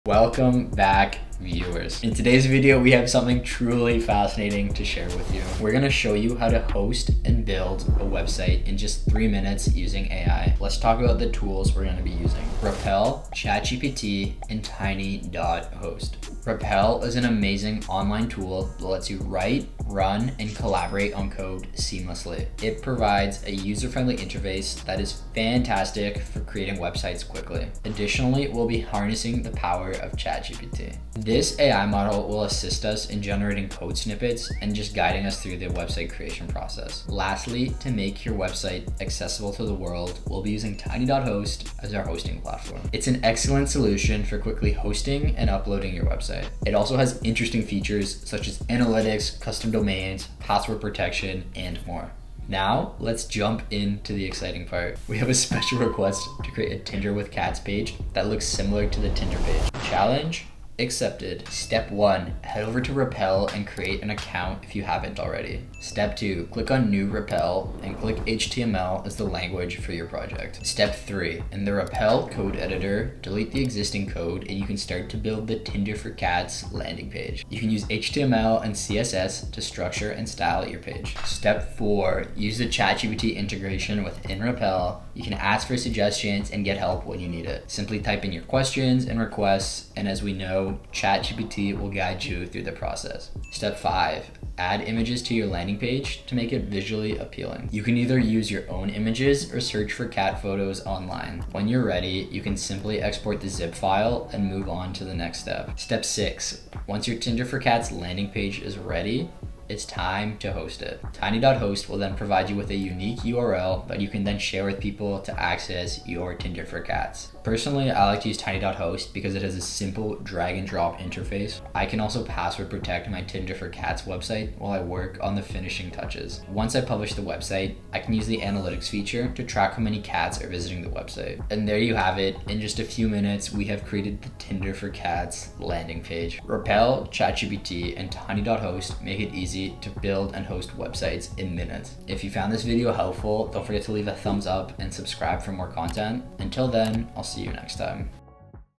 How to Build a Website in 3 Minutes Using ChatGPT
We're going to show you how to build and host a website in just three minutes using AI. Yes, you heard it right! It's time to discover the power of ChatGPT website creation.